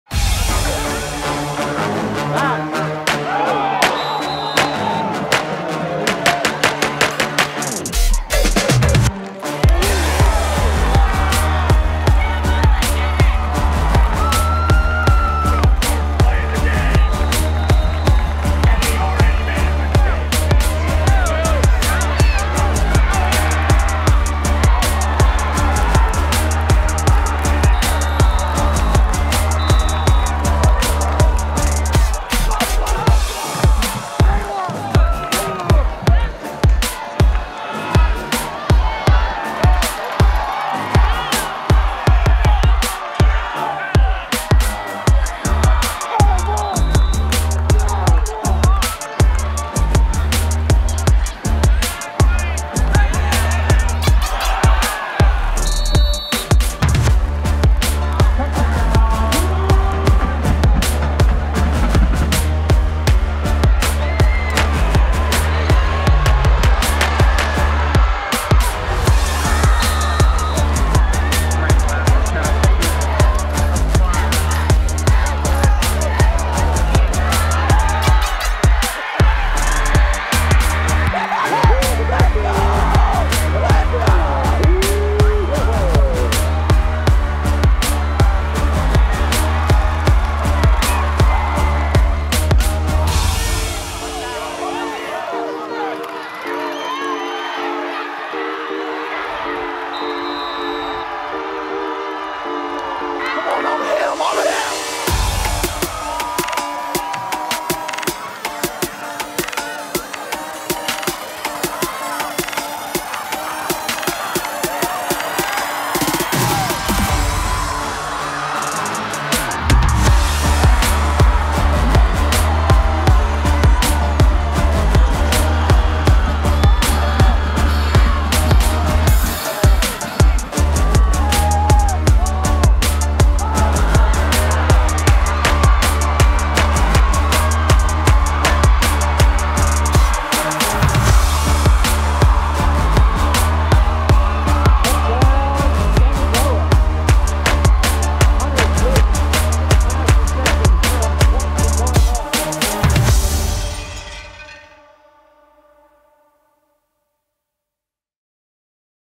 Storied Rivals' Week 5 Top Plays [Football 2022]
Check out this week's top plays from our week FIVE coverage (from just the 21 games we filmed) across Ohio. Did your team make the top ten!?